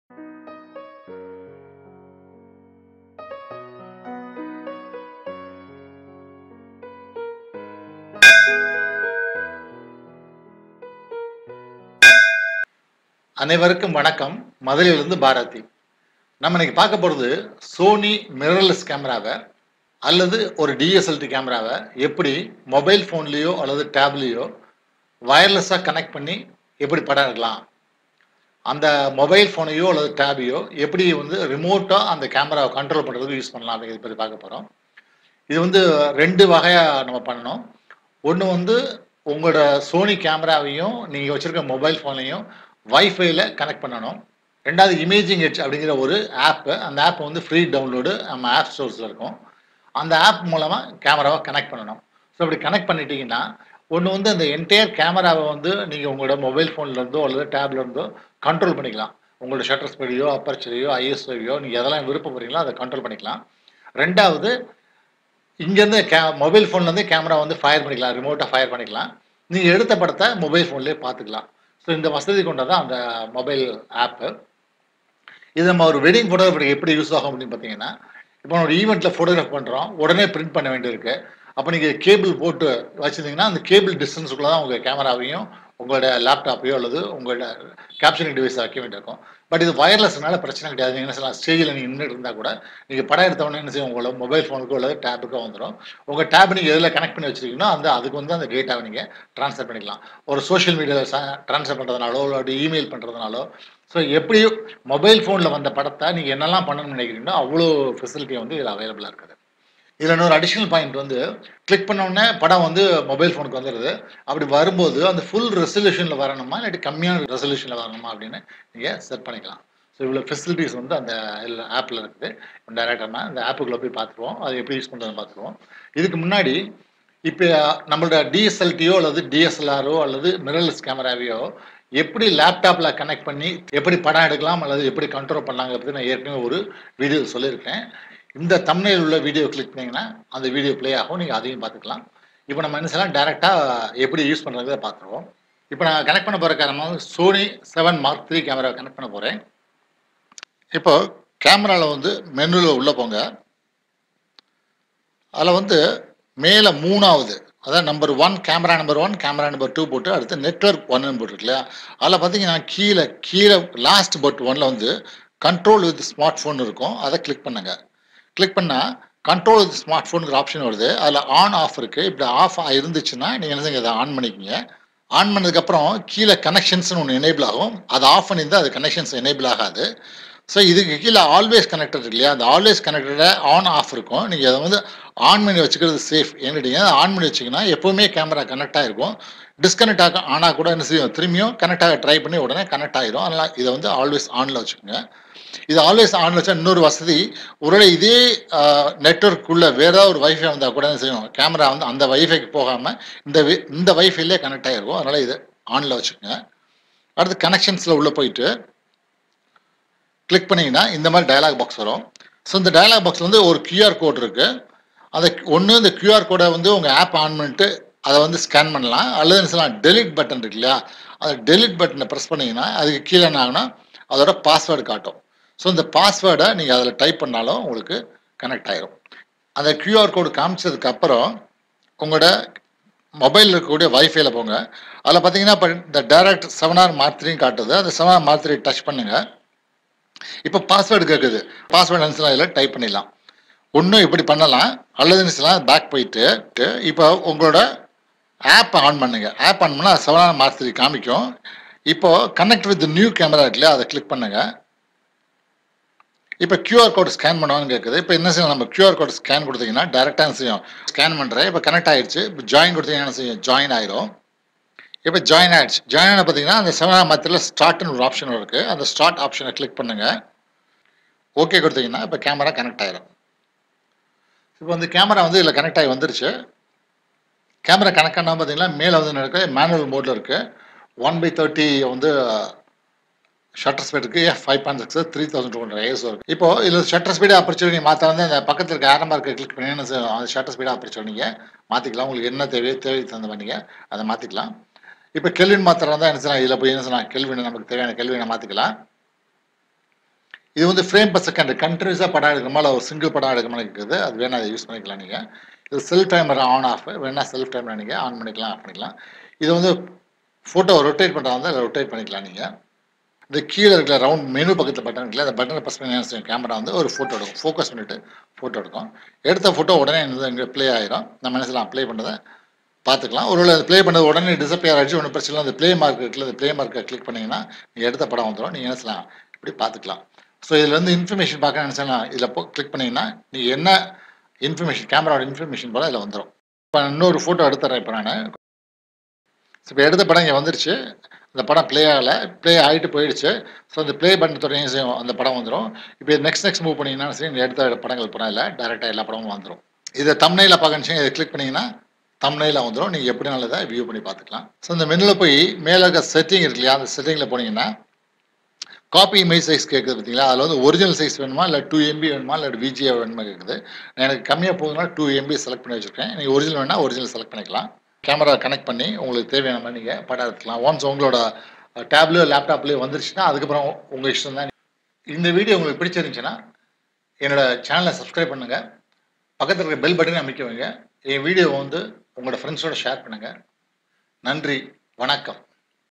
Sony Imaging Edge Mobile App - சோனி இமேஜிங் Edge மொபைல் பயன்பாடு
How to control sony Mirrorless camera with your mobile using Imaging Edge app  -  உங்கள் மொபைலில் சோனி கேமராவை  இணைத்து   படம் எடுப்பது எப்படி - தமிழில்